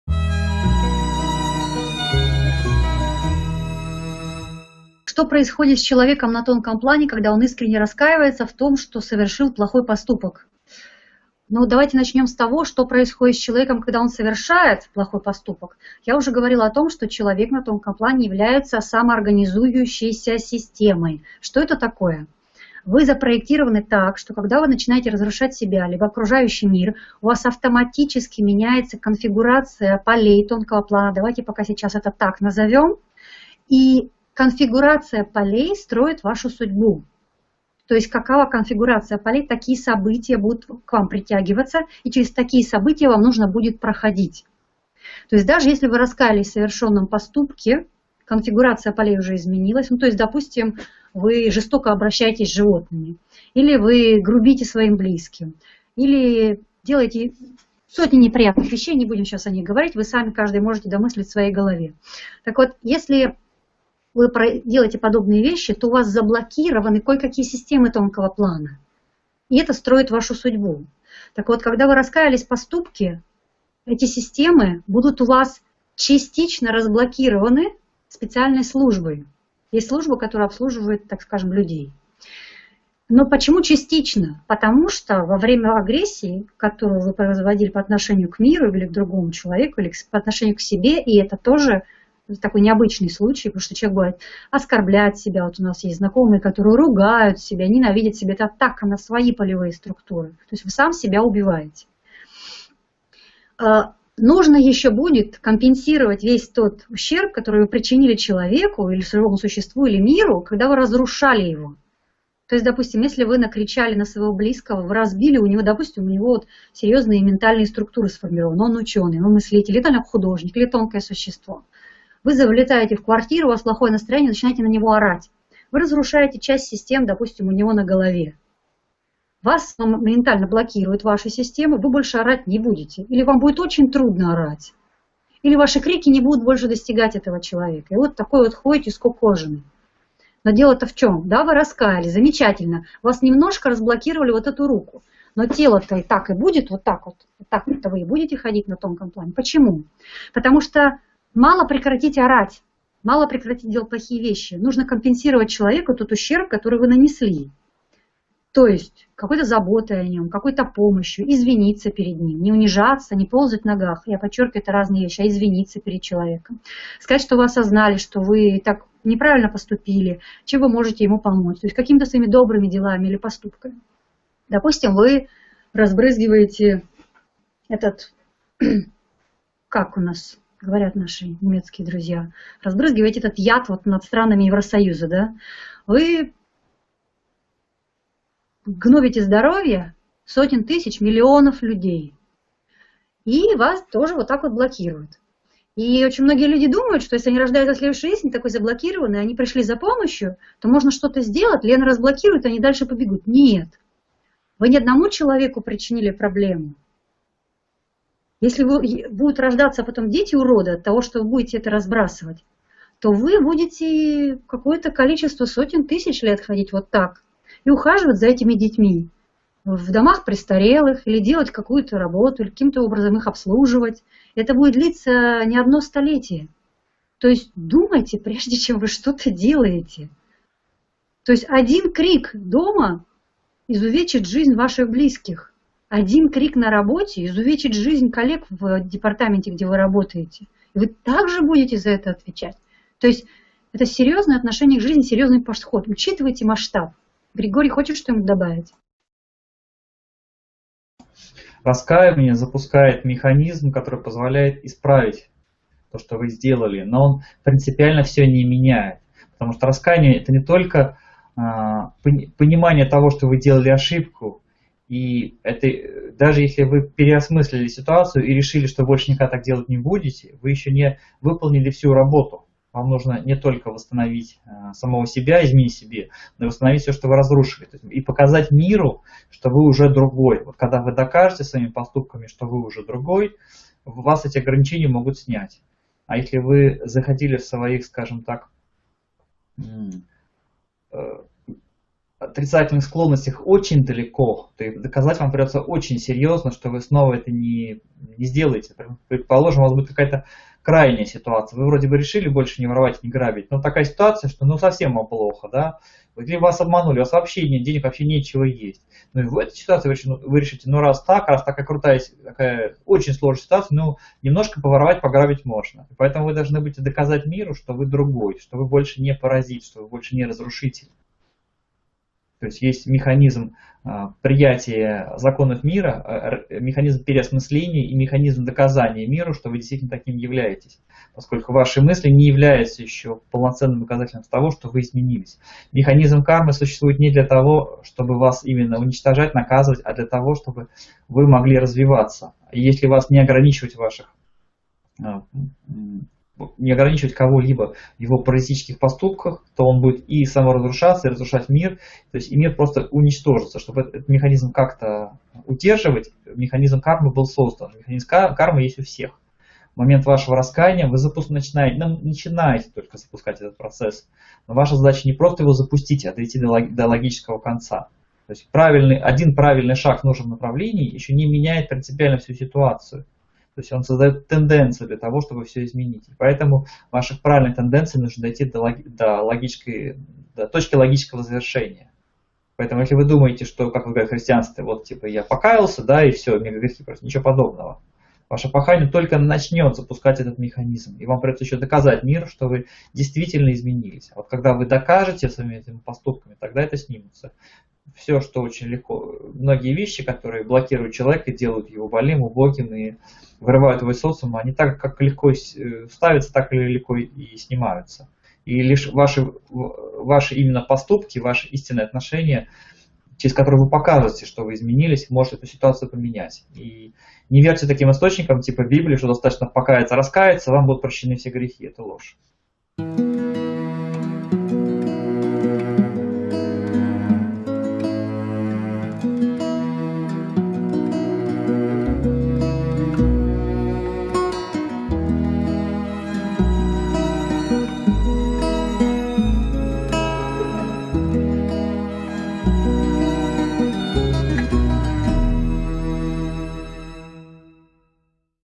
Влияние Поступков На Судьбу
Что происходит с человеком на тонком плане, когда он искренне раскаивается, что совершил плохой поступок? На вопрос участника онлайн-встречи отвечают Елена, целительница, и Григорий, эзотерический психолог.

* запрещены в России

Видео №3146